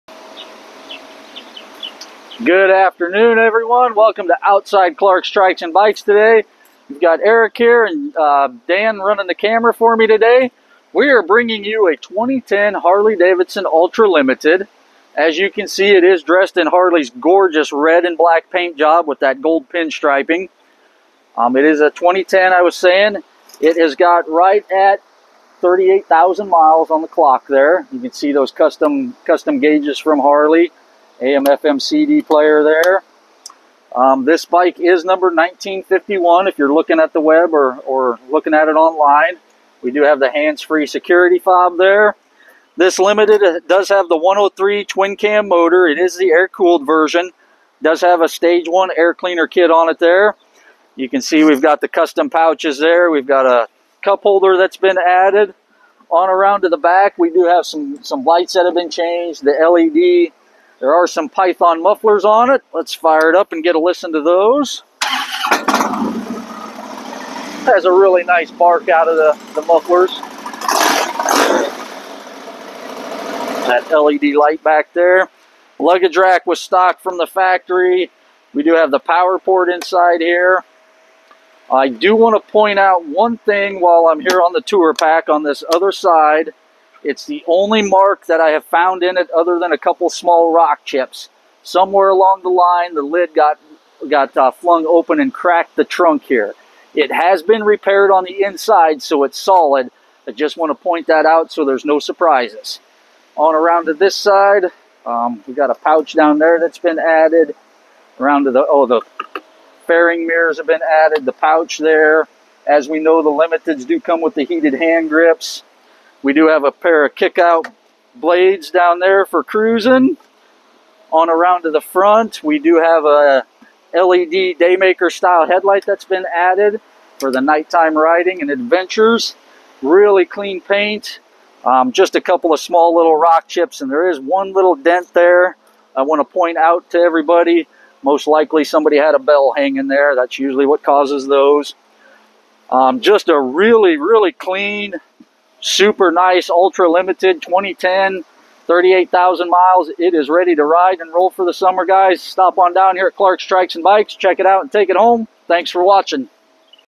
#1951 2010 Harley Davidson FLHTK Ultra Classic
Nice 2010 Harley Davidson FLHTK red black with only 37k for miles only $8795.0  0 OTD